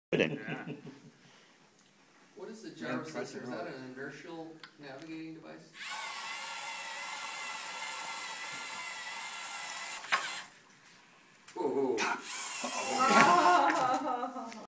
GyroWorks
This group managed to make their turn using the Gyro sensor but they had to turn to 45 degrees  instead of 90 to make the 90 degree turn, and we're not sure why.